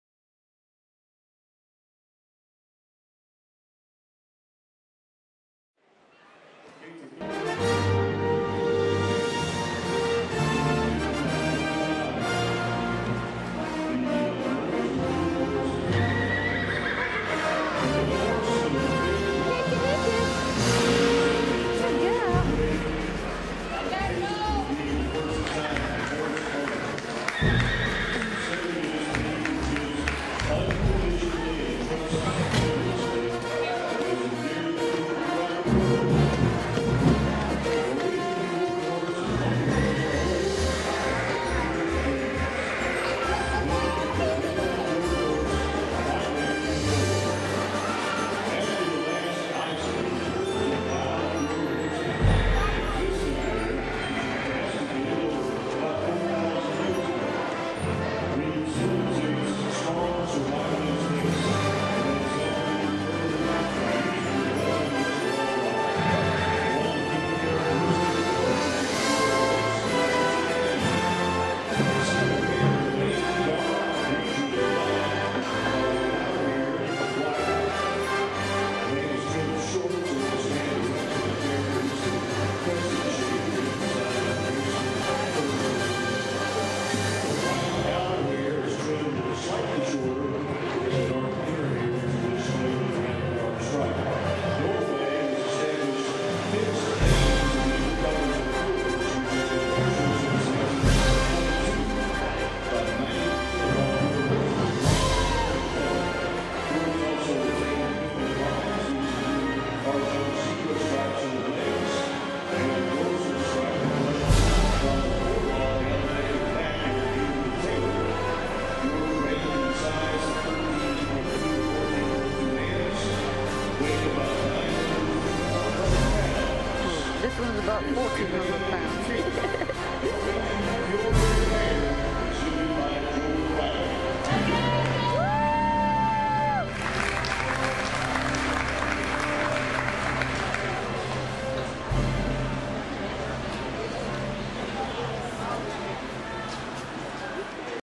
Joel and Cakelicious breed parade 2012
Joel and Cakelicious riding in the breed parade at the 2012 River Region Horse Councel expo, in Alabama. Cake (reg name. MAB Kate) is a 13 year old Norwegian Fjord mare.